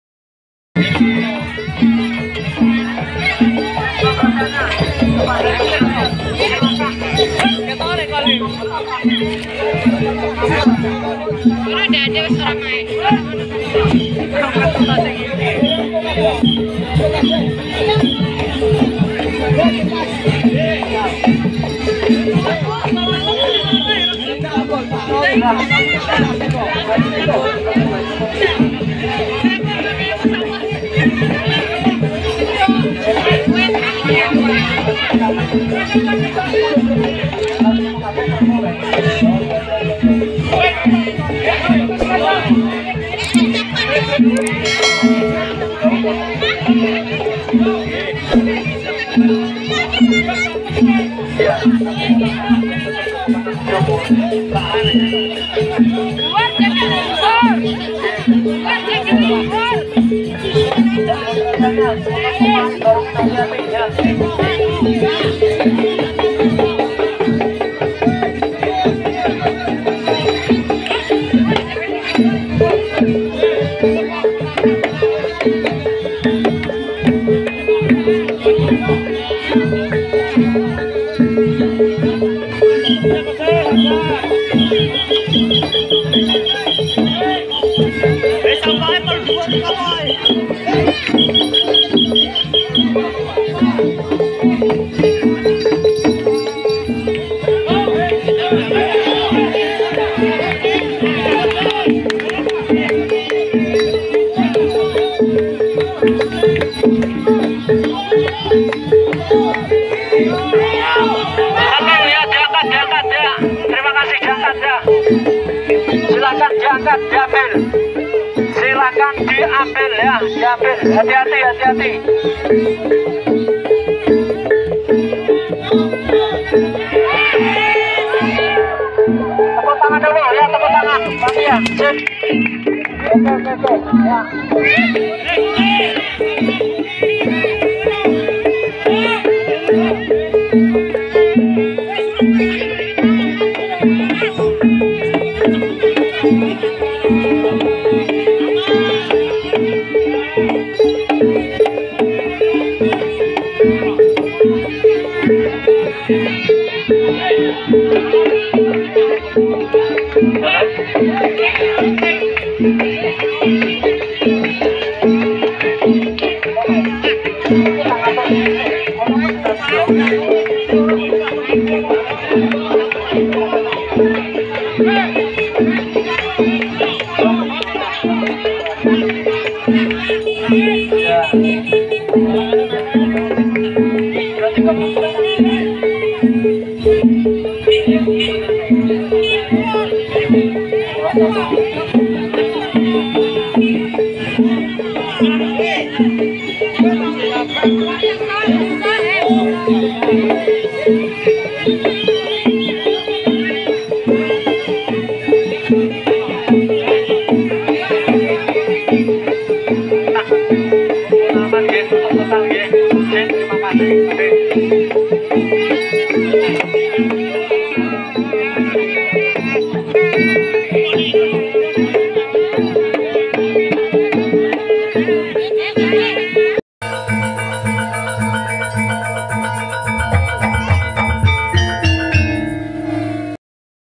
Meron Sukolilo 2022 Paling Spektakuler
Tradisi Meron Sukolilo Pati Jawa Tengah 2022 Paling Spektakuler sepanjang masa, begini suasana pemasangan meron sukolilo terbaru setelah 3 kali diterjang badai kopet akhirnya di gelar kembali dengan suasana lebih meriah, meron ini punya pegawai desa bapak Sutrisno, setiap perangkat di desa sukolilo ini mengeluarkan gunungan meron,

meron sukolilo 2022, PUNCAK GEBYAR MERHON 2022 kali ini berjalan ramai pol sangat meriah di iringi gamelan barongan pati,